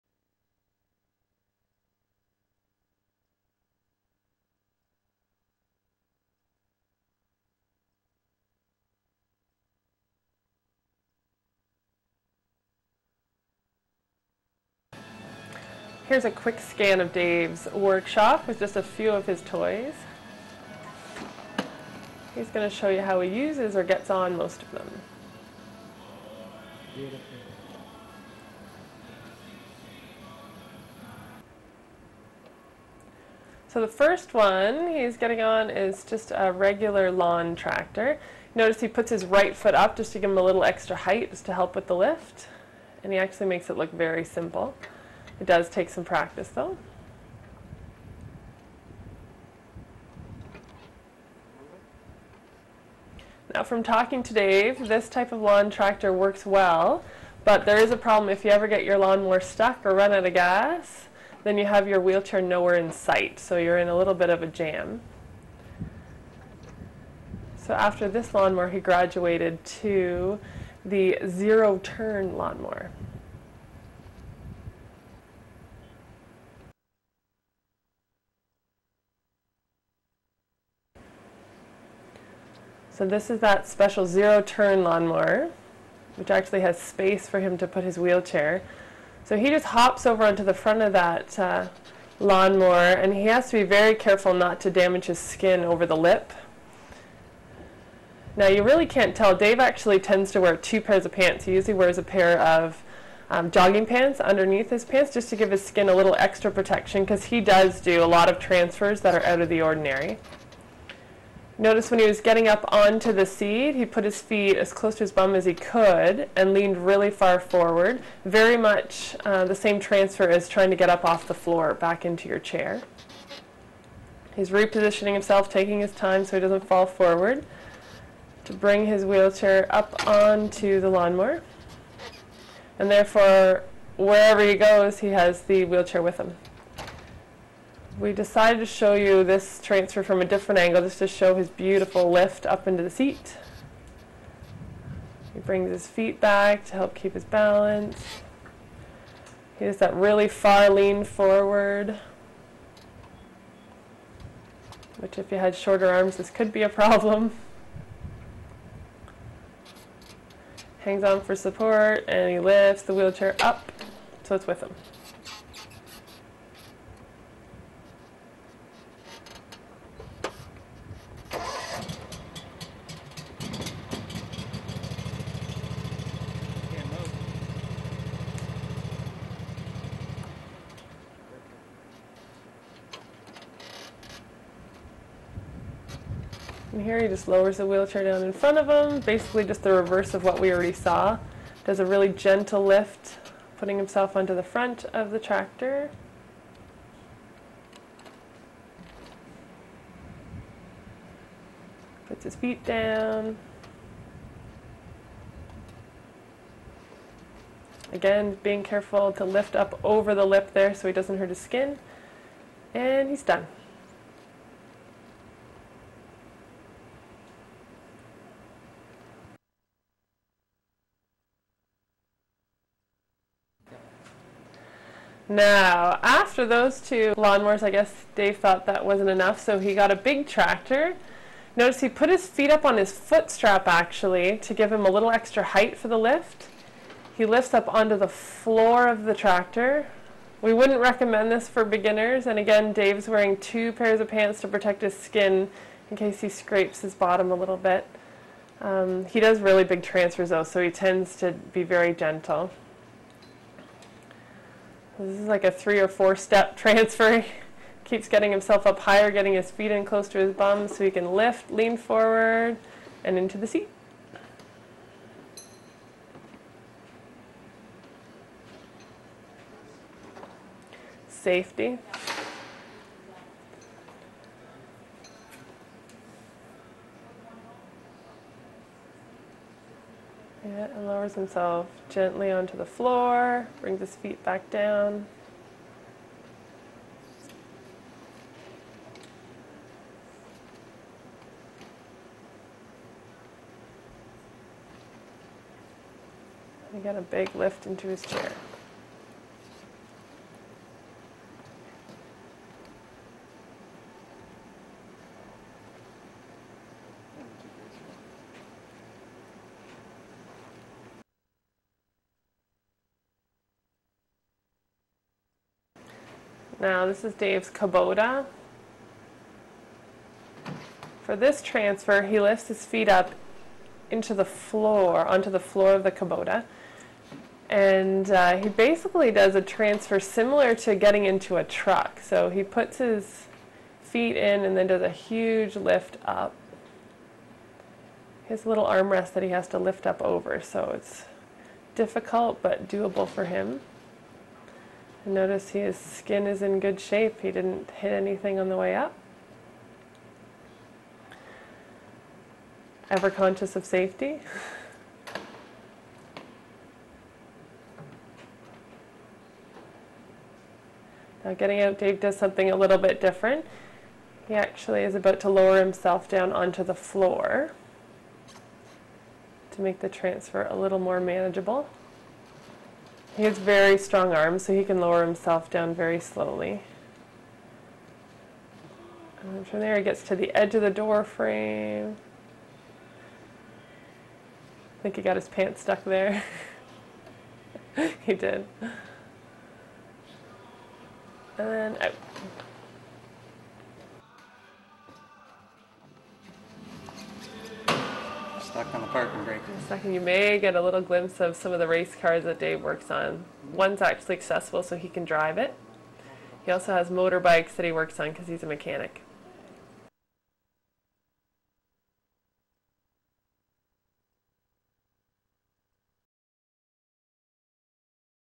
5Utility Vehicle Transfers CCTV.mpg
Spinal Cord Injury Rehabilitation Program, SCI Transfers & Wheelchair Skills, Utility Vehicles Transfers